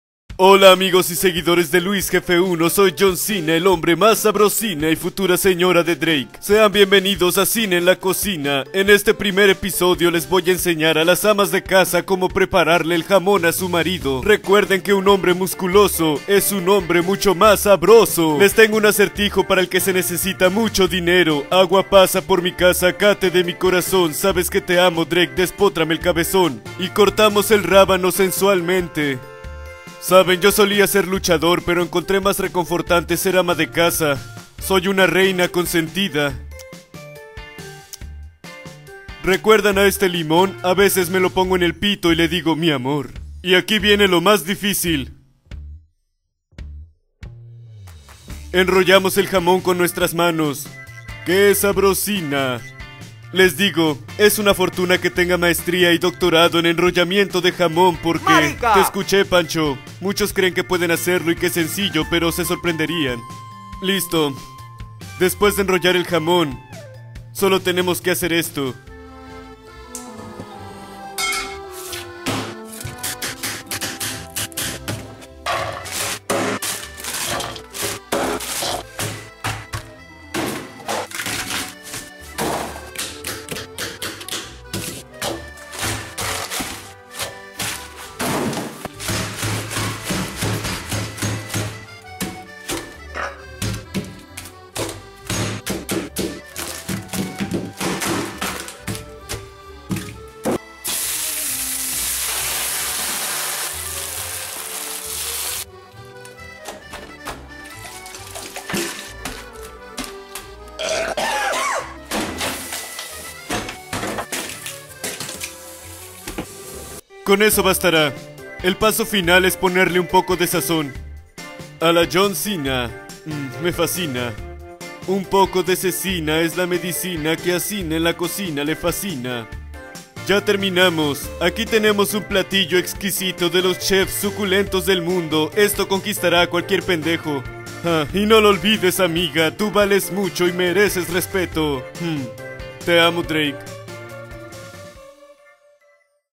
Preparando Jamón con John Cena en la CoCena - Luisjefe1Vlogs
Para todas las amas de casa, John Cena les muestra cómo prepararle el jamón a su marido.